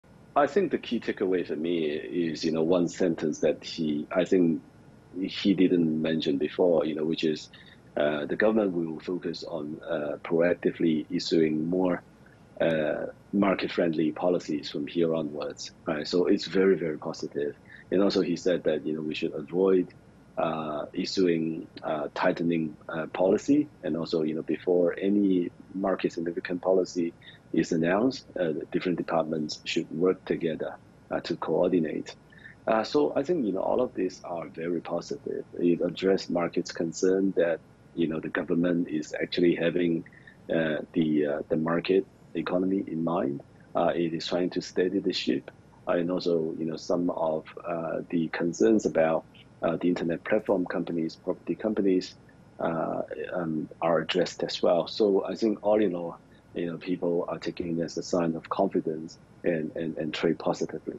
Strategist: Chinese government allays concerns about internet and property companies
China's State Council on Wednesday mentioned the government will roll out more market-friendly policies and avoid tightening policies. The announcements addressed some of the market's concerns about the internet platform and property companies and will help to steady market sentiment, says Hong Hao, chief China strategist at Bank of Communication International Holdings.